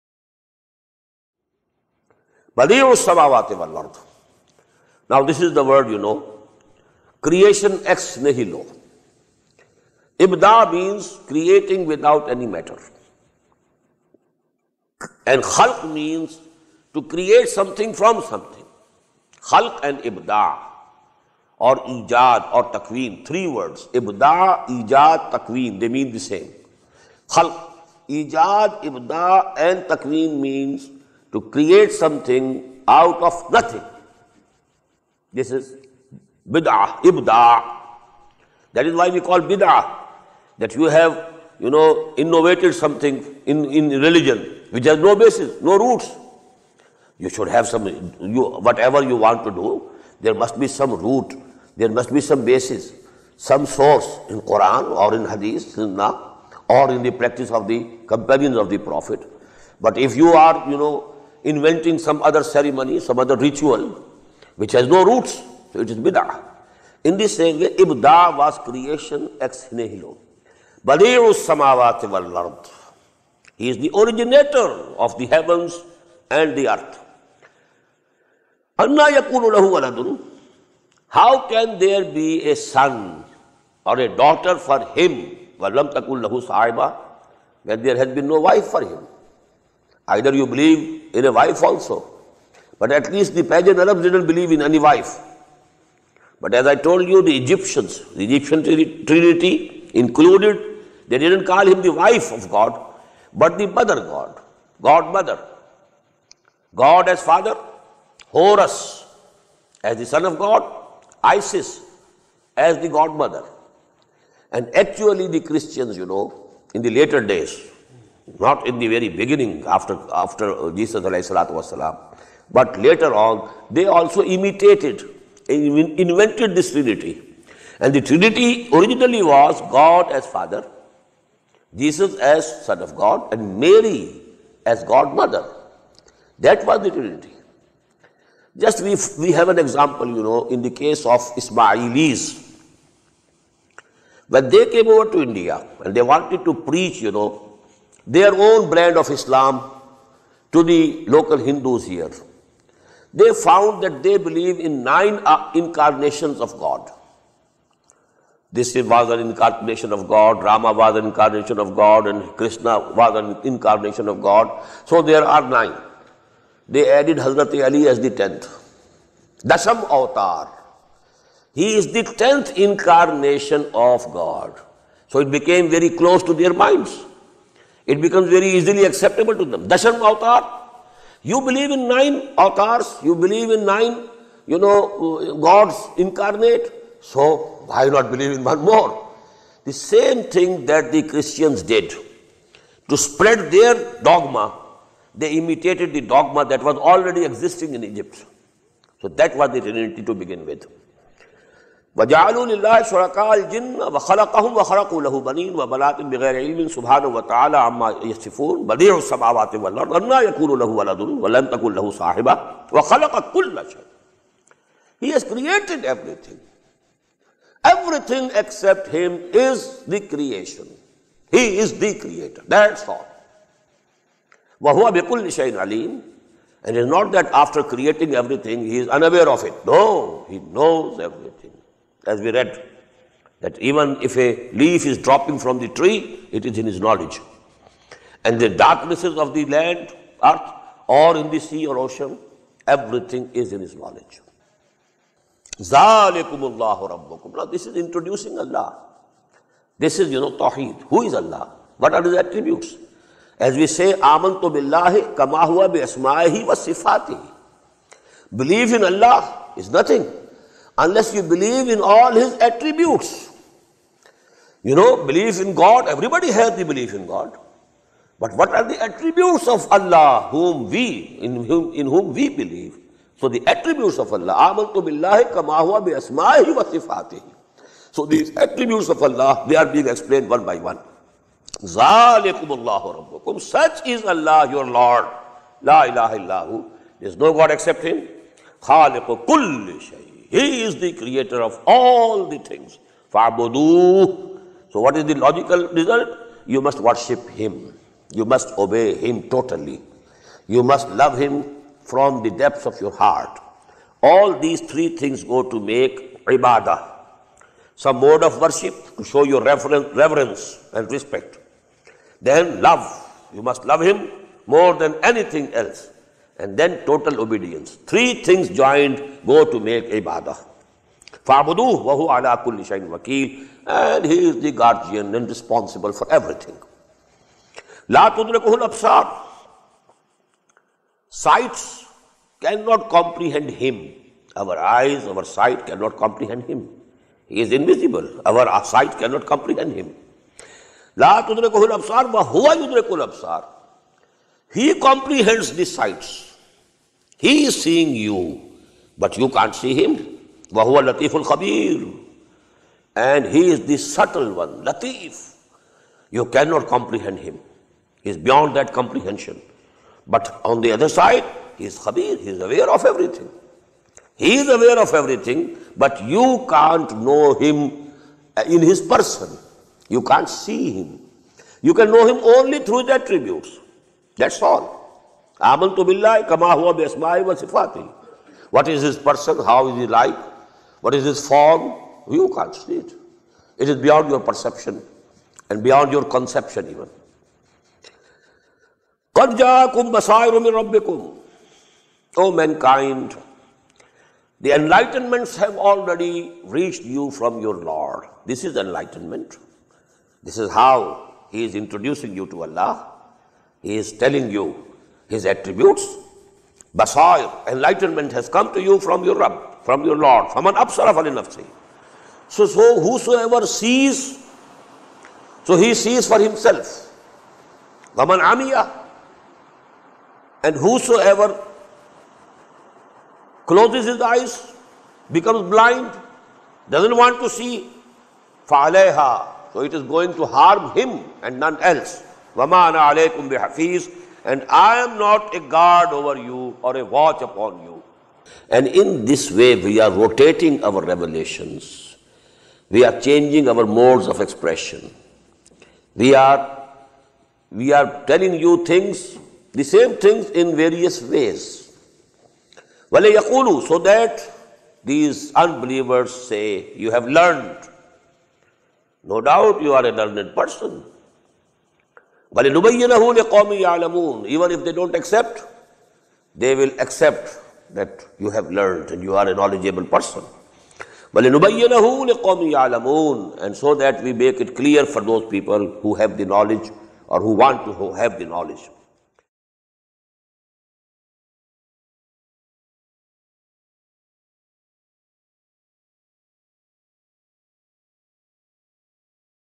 Surah An'aam Ayah 101-105 Tafseer in English by Dr. Israr Ahmed
Bayan ul Huda, Surah An'aam Ayah 101-105 Tafseer in English by Dr. Israr Ahmed

TOPICS COVERED IN THESE AYAHS:
1. ALLAH is the creator and aware of everything.
2. There is no God but ALLAH, only worship Him.
3. Sights cannot comprehend ALLAH. Know Him through His attributes.

Narrated Anas:
The Prophet (ﷺ) used to take a bath with one Sa` up to five Mudds (1 Sa` = [??] Mudds) of water and used to perform ablution with one Mudd of water.